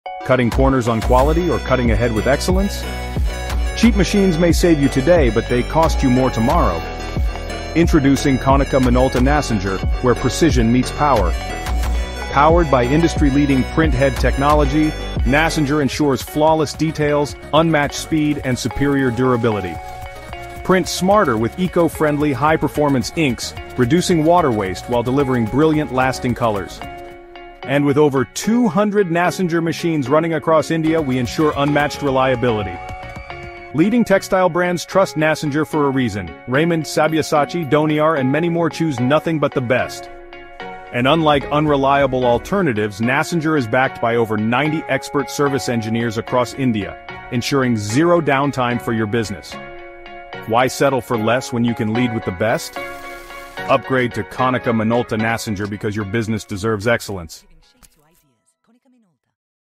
Konica Minolta Nassenger Series | India's Best Digital Textile Printer | Raymond, Donear Trusted
The Konica Minolta Nassenger series represents the cutting edge in digital textile printing technology. Designed for high-speed, high-quality textile production, Nassenger printers deliver exceptional performance, reliability, and versatility across a wide range of fabrics.

Engineered with Konica Minolta’s proprietary inkjet technology, these machines offer superior precision and vibrant color reproduction—ideal for meeting the fast-paced demands of modern textile manufacturing.

Key Features:
High-Speed Output: Capable of printing at industrial production speeds without compromising print quality.
Exceptional Image Quality: Equipped with high-resolution, multi-drop gray scale printheads for smooth gradations and rich, vivid colors.
Eco-Friendly Ink System: Uses water-based inks with low environmental impact, supporting sustainable textile production.
Versatile Fabric Compatibility: Suitable for a variety of materials including cotton, silk, polyester, and blended fabrics.
Advanced Automation: Integrated features for automatic cleaning, head maintenance, and precise media handling reduce downtime and boost productivity.
Ideal for fashion, interior decor, and soft signage applications, the Nassenger series helps manufacturers meet diverse market demands while improving operational efficiency.